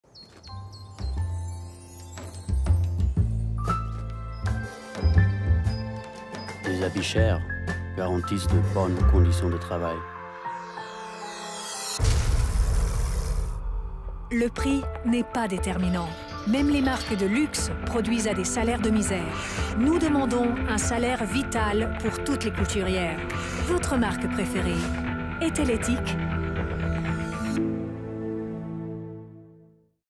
Les vêtements chers sont forcément produits dans des conditions éthiques. Vérité ou idée reçue ?
Les vêtements chers sont confectionnés dans des conditions éthiques ? Faux ! Même les marques de luxe produisent à des salaires de misère. Et ta marque préférée ? Pour en savoir plus